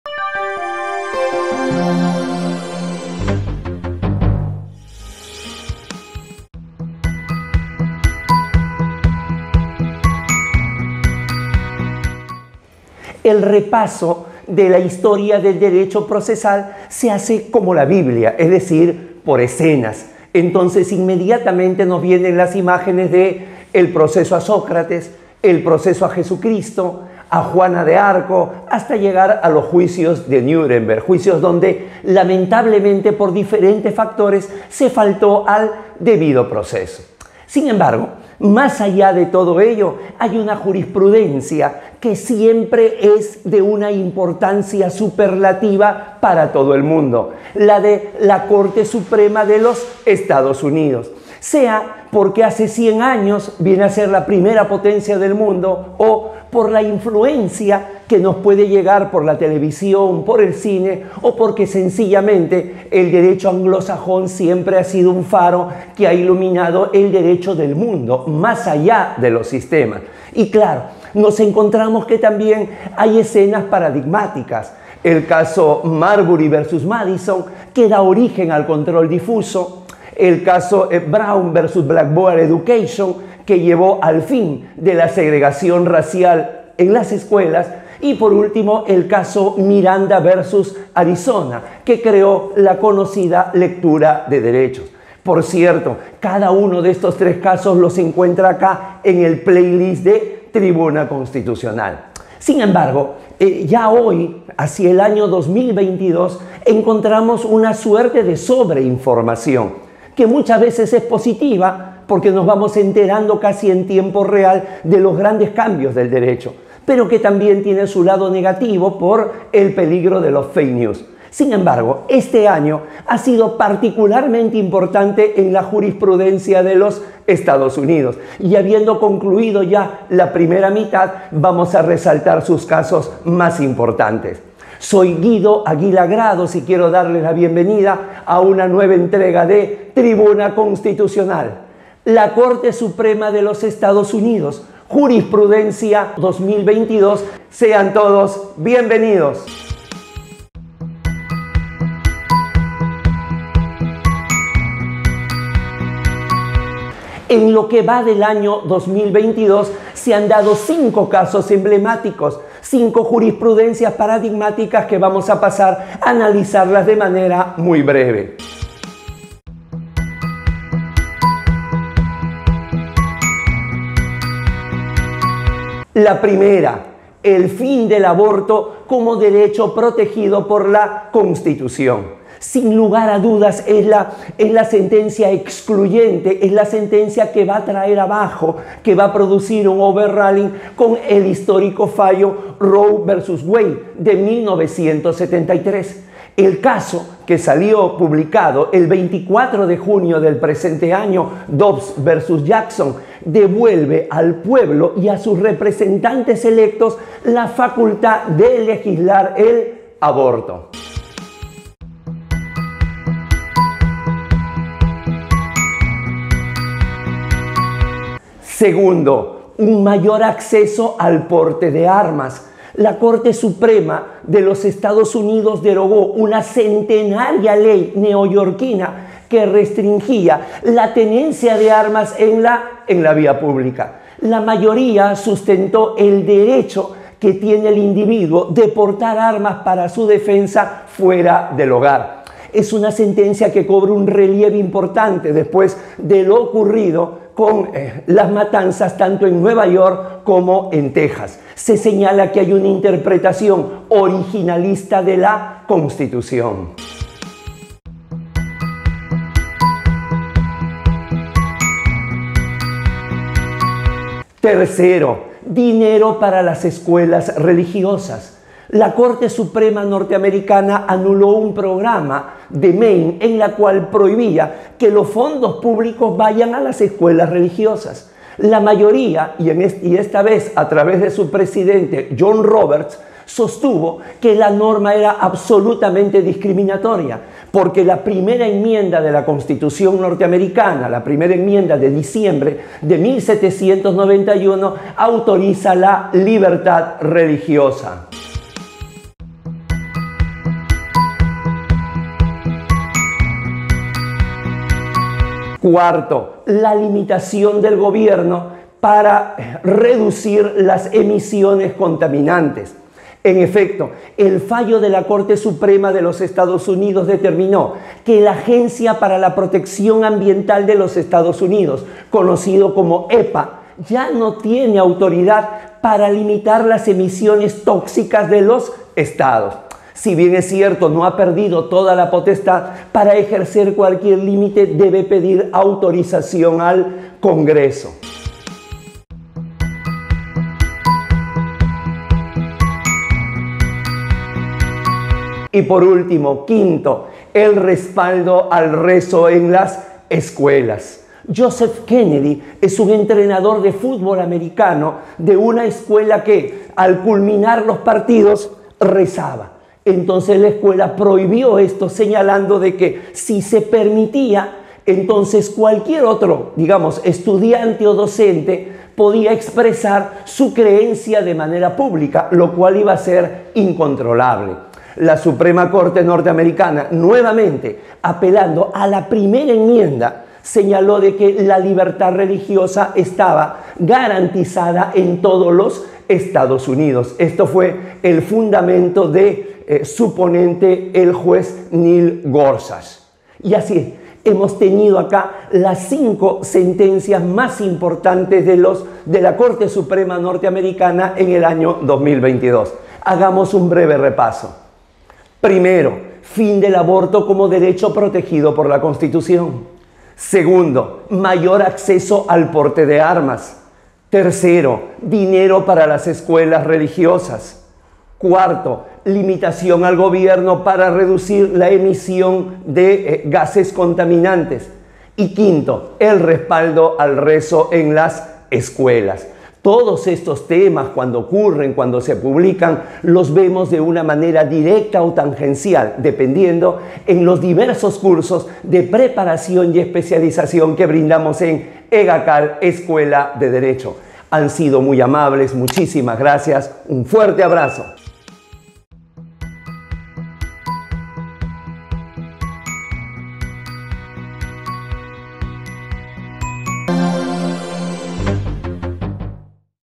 LA CORTE SUPREMA DE EE. UU.: Jurisprudencia 2022 - TC # 315
La Corte Suprema de los Estados Unidos es la más importante del mundo con respecto a la trascendencia de sus fallos. En la presente entrega resumimos las cinco sentencias más notables de la primera parte del año 2022.

🔴𝐏𝐀𝐑𝐀 𝐂𝐎𝐍𝐒𝐔𝐋𝐓𝐀𝐒
📌⚖ Cursos especializados y asesorías académicas